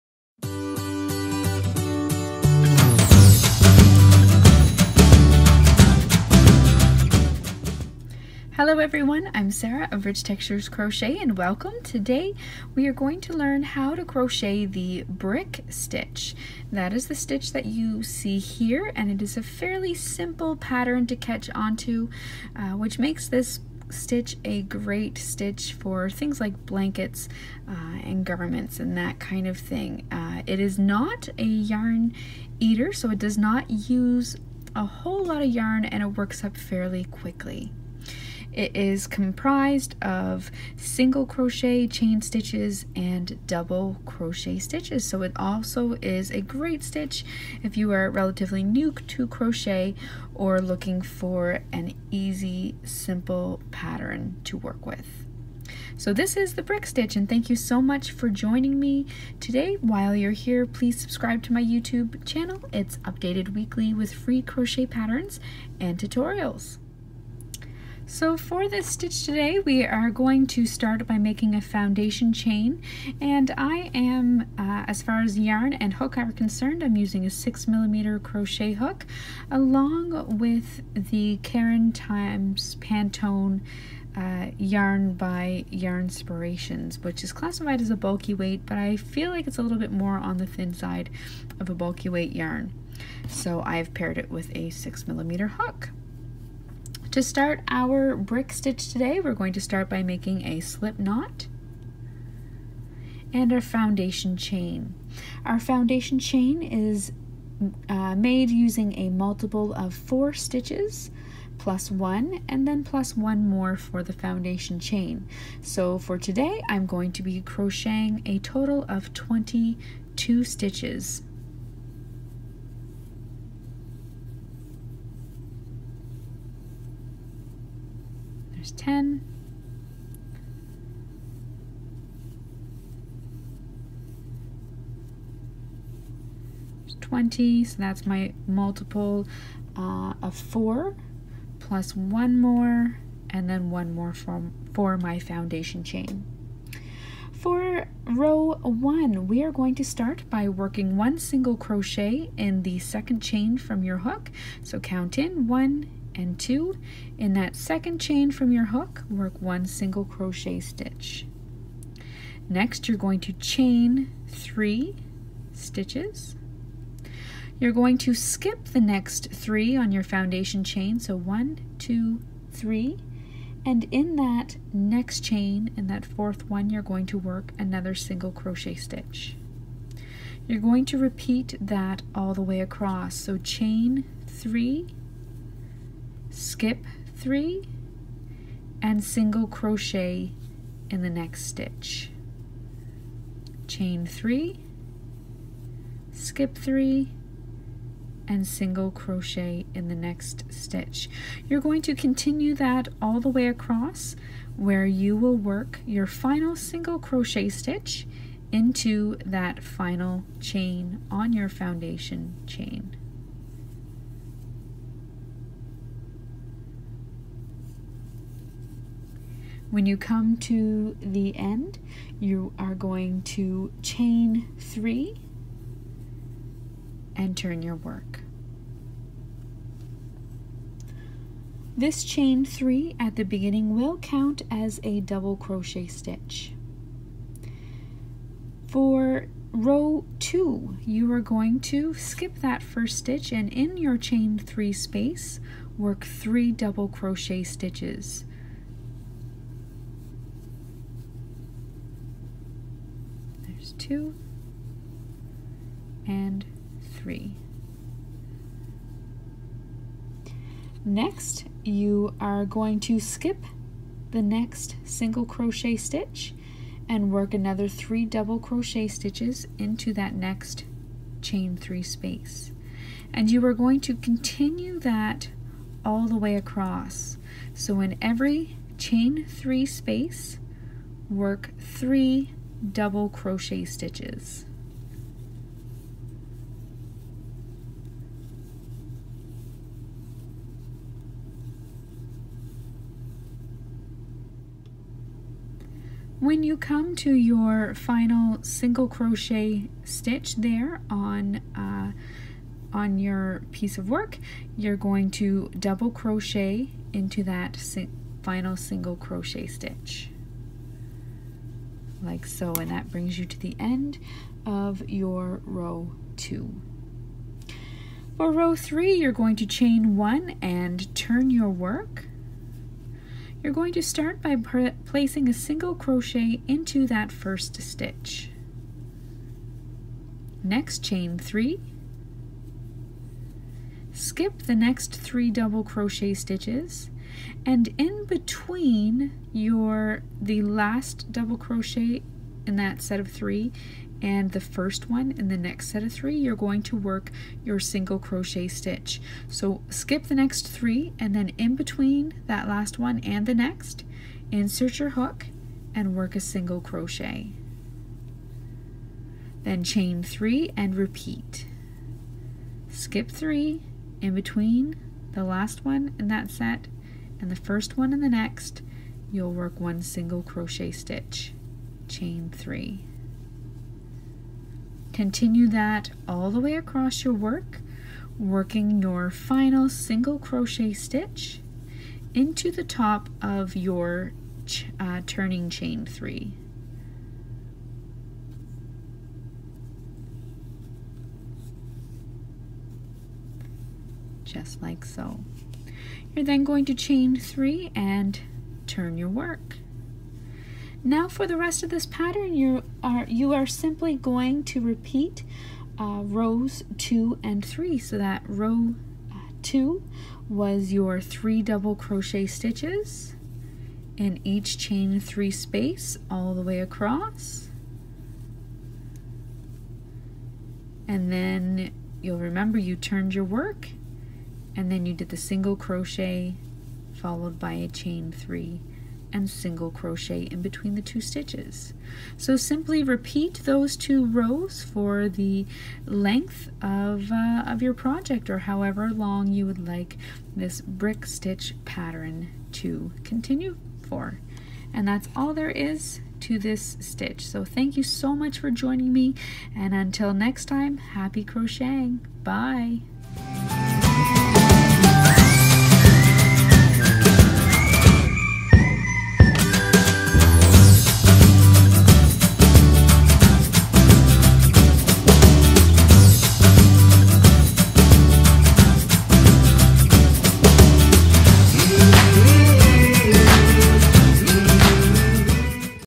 Brick Stitch | How to Crochet
Today we are going to learn how to crochet the brick stitch.  This stitch works up quickly and does not use a lot of yarn.  It's great for blankets and larger projects.

Sarah